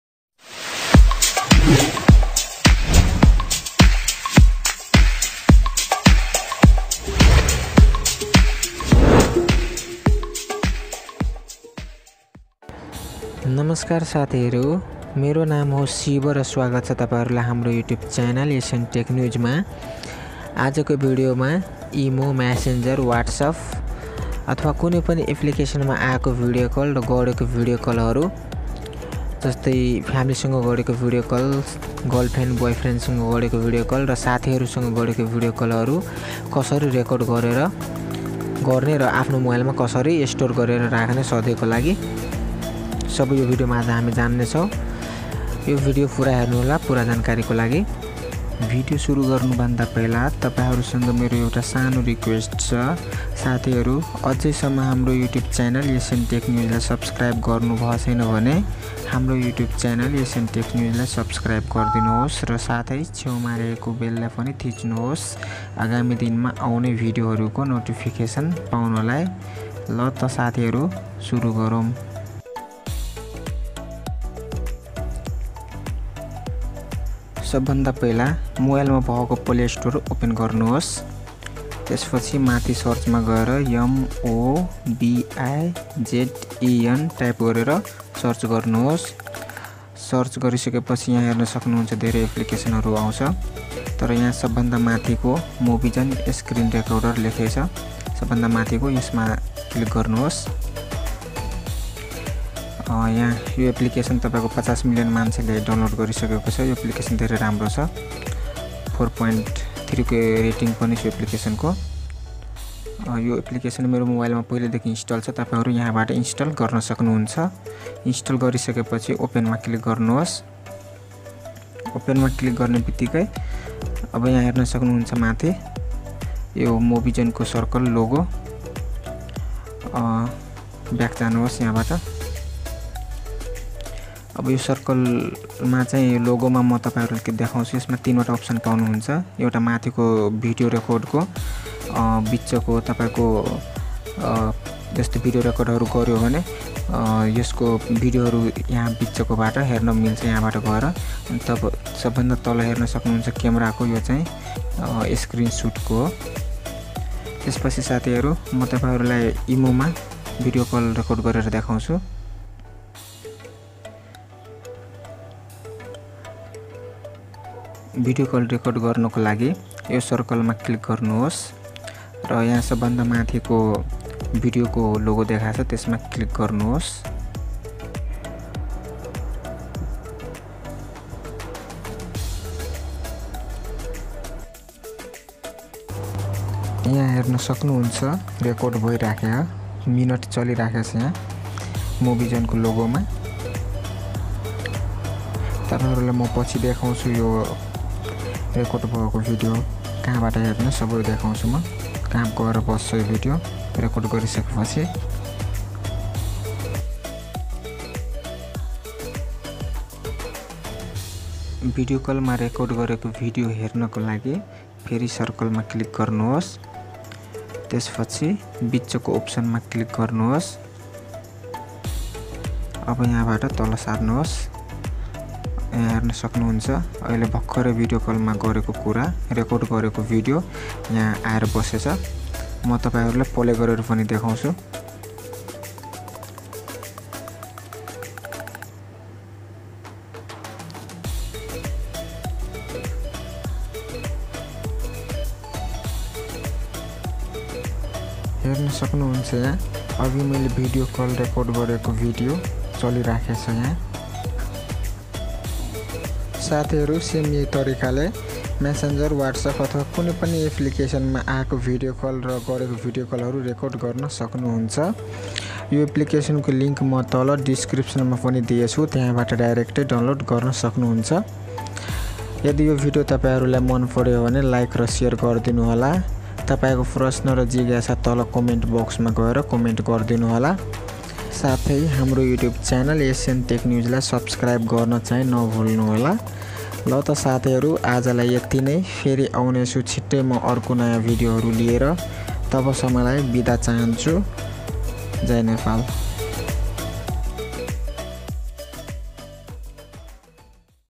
How to Record Video call on IMO, Facebook, What'sapp, Skype on Mobile | Nepali | by SN-tech News
How to Record Video call on IMO, Messenger, What'saap, Skype and all apps on mobile

साथीहरु. यो video मा IMO, Messenger, What'saap, Skype र अरु सबै Apps हरु माअाएको अथवा गरेको video Call हरु कसरी रेकर्ड गर्ने. सवै यो video मा देखाएको छु। पुरा जानकारी को लागी यो video पुरा हेर्नुहोस् ।साथीहरु यो video मन पर्यो भने Like र Share गर्दिनुहोला र तपाईको विचार अथवा कुनै प्रशन तल कोमेन्ट बोक्स मन गएर कोमेन्ट गर्नुहोला ।